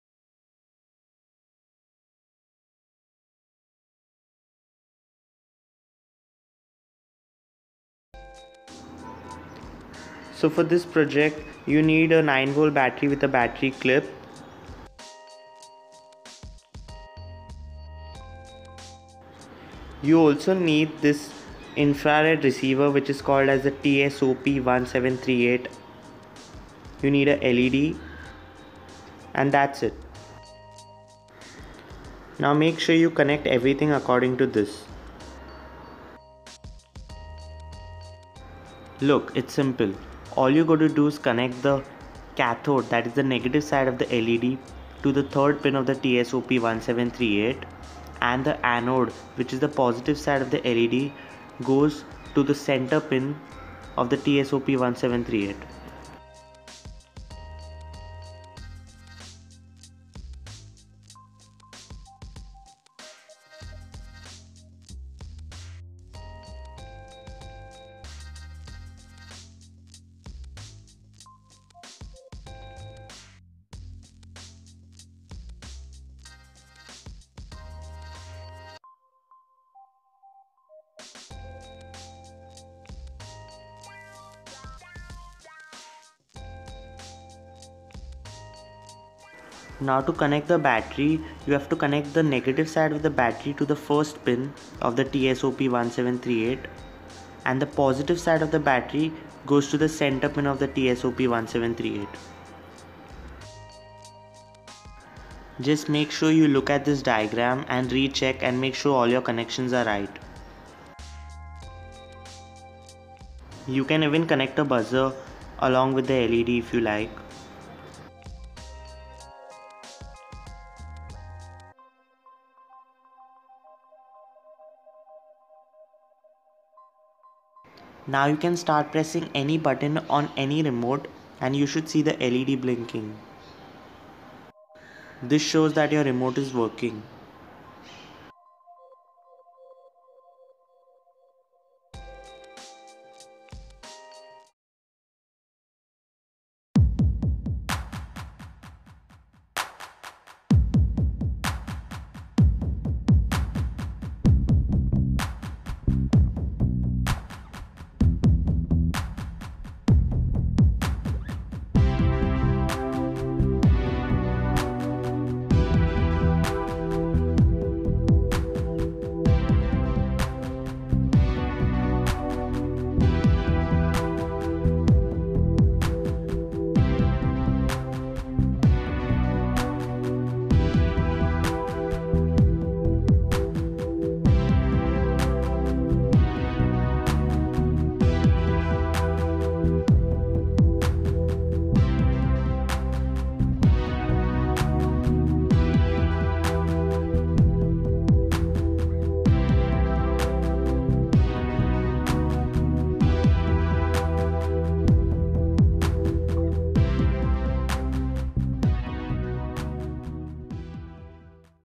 How to make an "IR Remote Tester" Circuit [With & Without Breadboard]
In this video I will show you how you can make an infrared remote tester circuit either with a breadboard or without a breadboard.